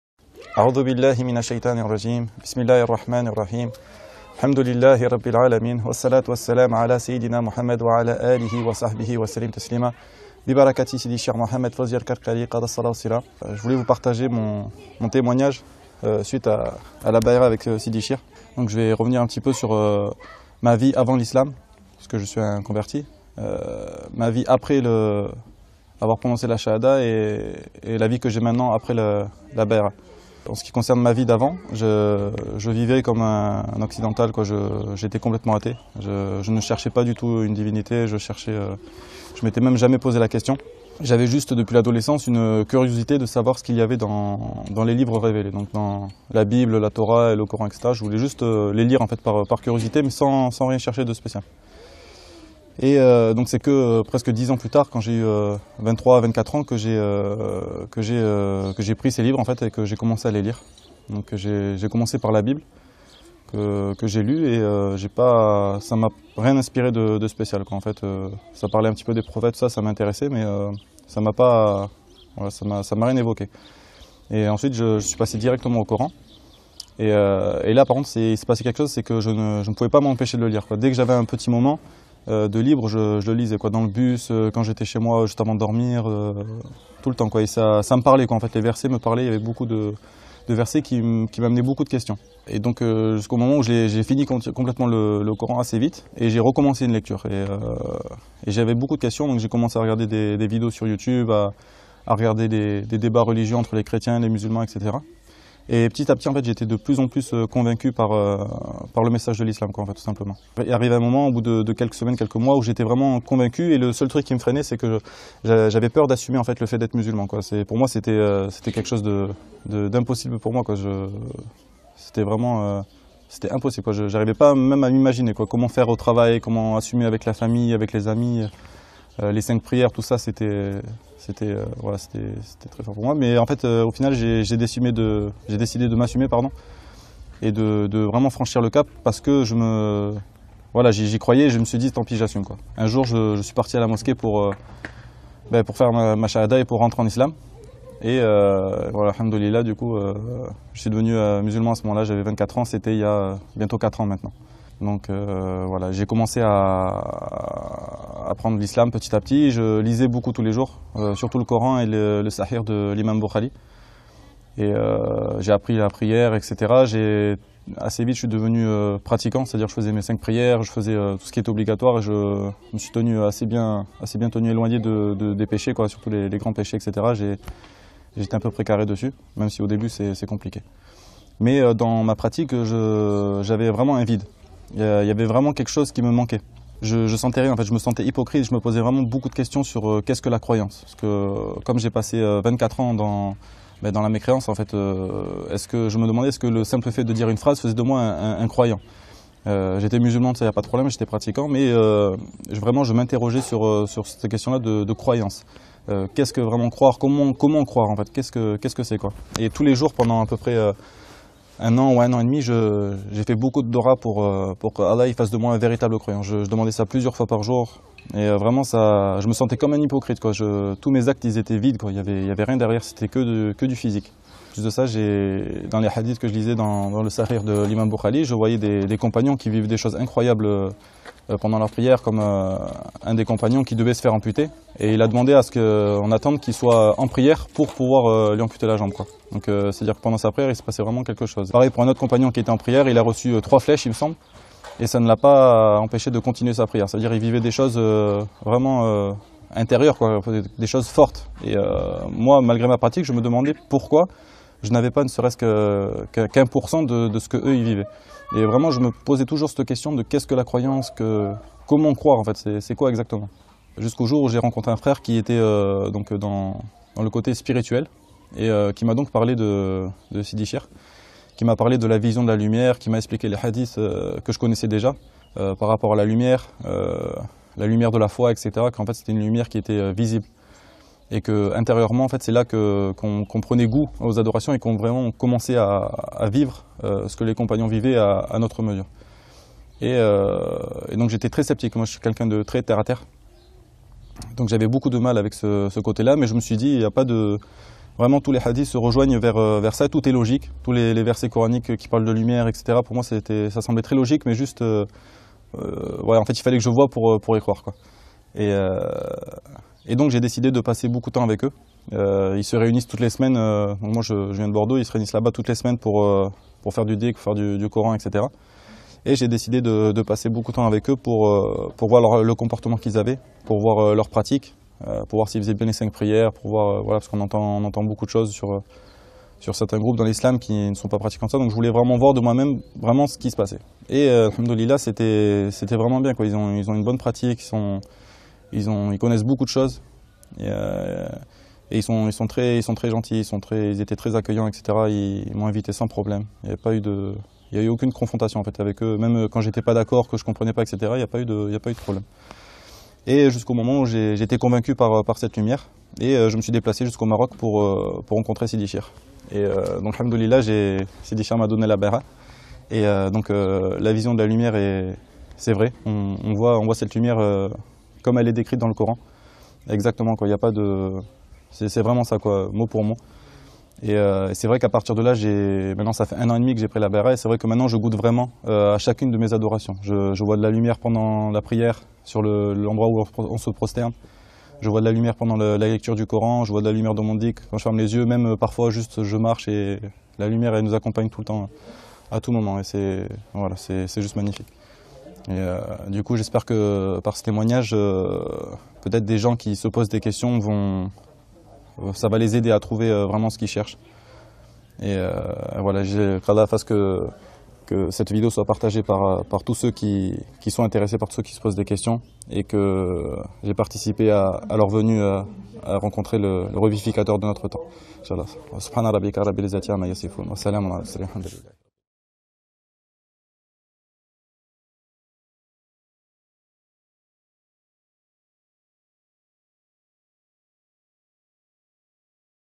From atheism to Islam (sufism) - Maxime from France
How Maxime went from atheism to Islam and how he discovered sufism.